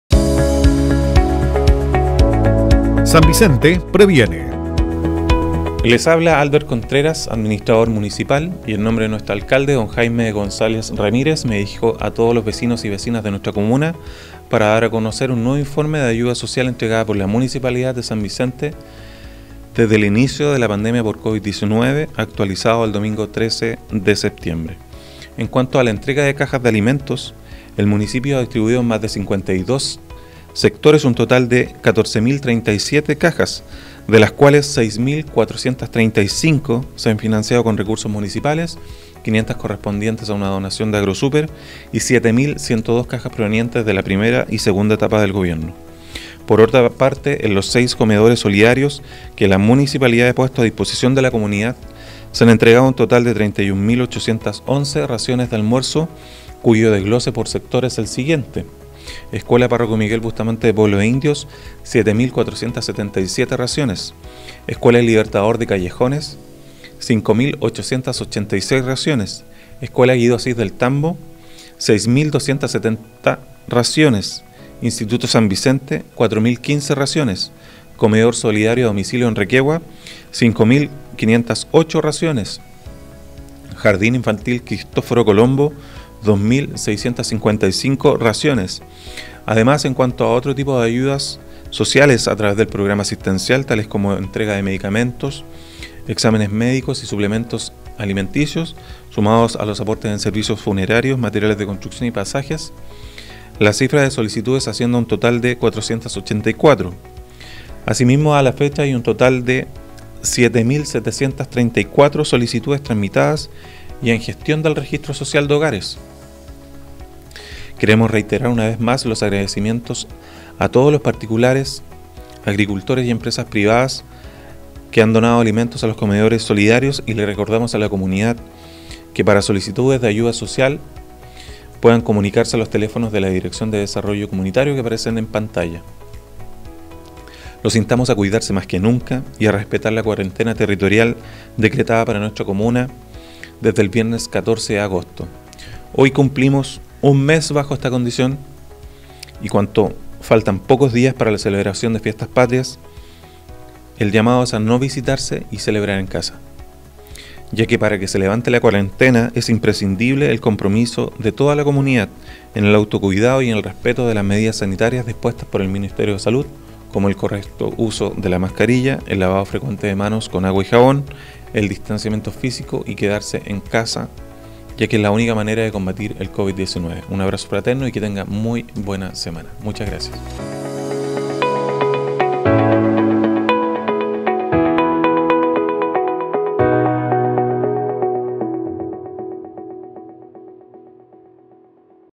BALANCE SOBRE ENTREGA DE AYUDA SOCIAL ACTUALIZADO AL 13 DE SEPTIEMBRE
El administrador municipal, Albert Contreras Aranda, en representación del alcalde, Jaime González Ramírez, realizó un nuevo balance de la entrega de ayuda social, correspondiente a cajas de alimentos y raciones de almuerzos en comedores solidarios, además de las cifras con respecto a los aportes del programa asistencial y solicitudes tramitadas y en gestión del Registro Social de Hogares, lo que ha permitido que nuestros vecinos y vecinas puedan postular y/o recibir los beneficios otorgados por el Gobierno durante la pandemia. Este informe está actualizado al 📅 domingo 13 de septiembre de 2020.

En dicho reporte, se da cuenta de las cajas de alimentos entregadas con recursos municipales ✅ (6.435), donación de Agrosuper ✅ (500) y fondos del Gobierno Regional✅ (2.102 / etapa 1); (5.000 / etapa 2) llegando a un total de ✅ 14.037 cajas a la fecha.

Por su parte, la suma de raciones de almuerzos distribuidos en los seis comedores solidarios asciende a las ✅ 31.811.

En cuanto a las ayudas sociales realizadas a través del Programa Asistencial, la cifra asciende a ✅ 484 solicitudes resueltas, distribuyéndose entre la entrega de medicamentos, exámenes médicos y suplementos alimenticios, además de los aportes en servicios funerarios, materiales de construcción y pasajes.

Finalmente, se da cuenta de las ✅ 7.734 solicitudes tramitadas y en gestión del Registro Social de Hogares.